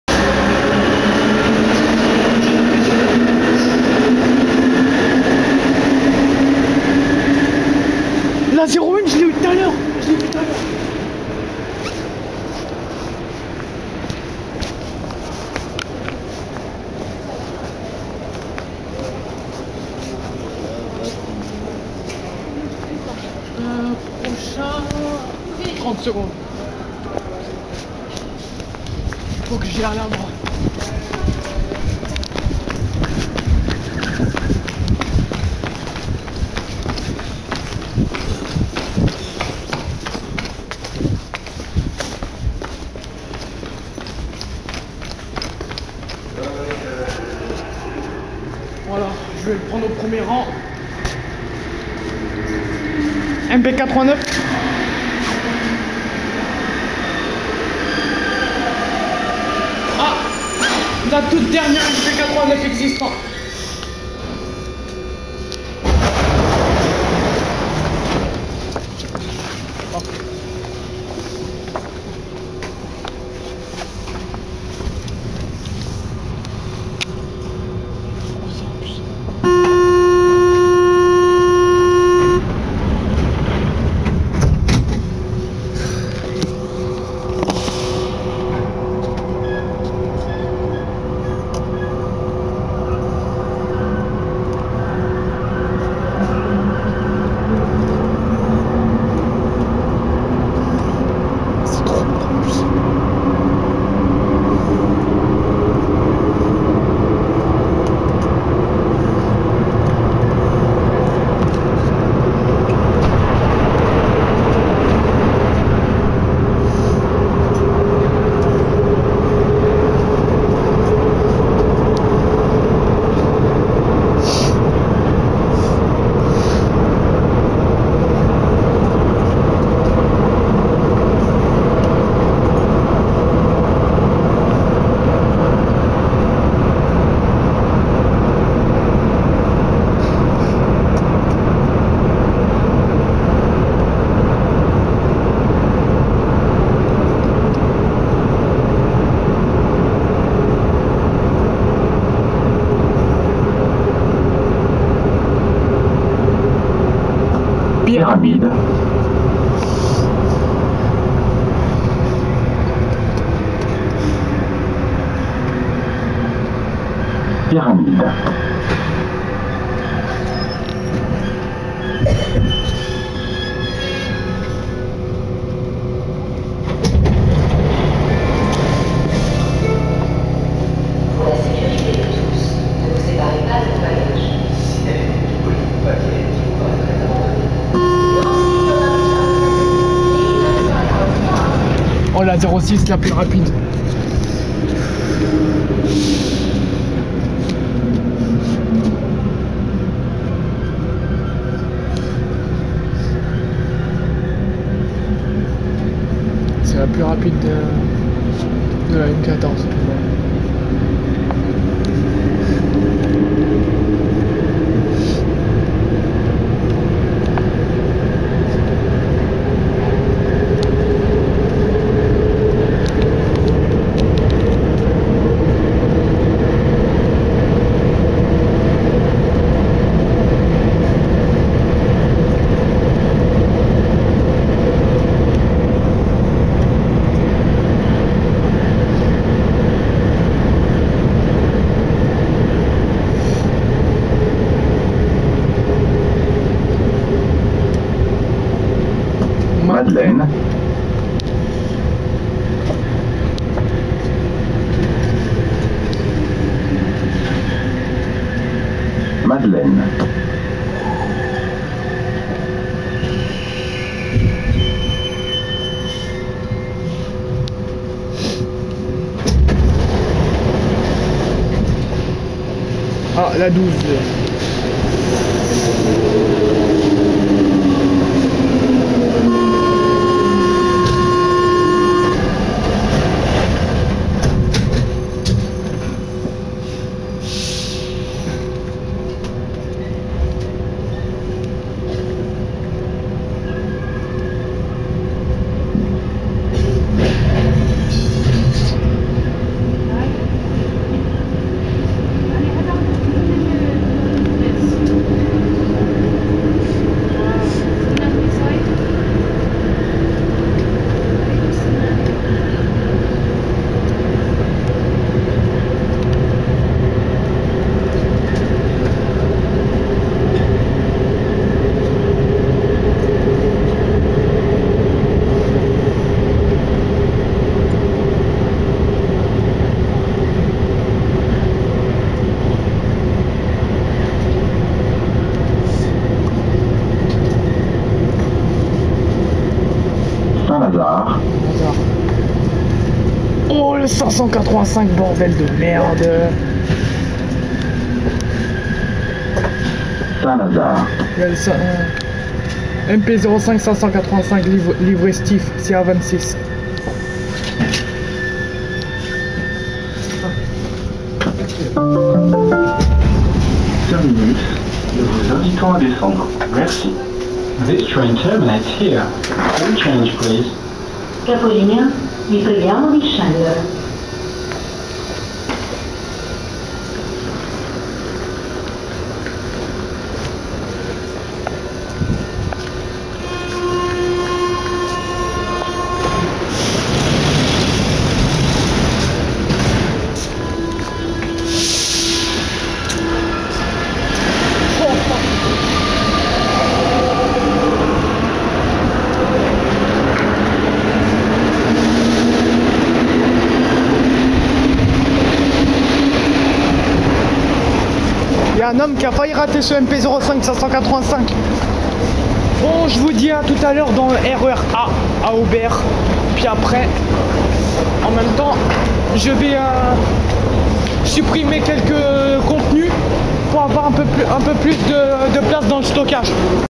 2015 : Ligne 14 Départ du CA01 + trajet entre Chatelet et Saint-Lazare Avec la CA21
avec départ du MP05 livrée Stif 585 CA26 a Saint-Lazare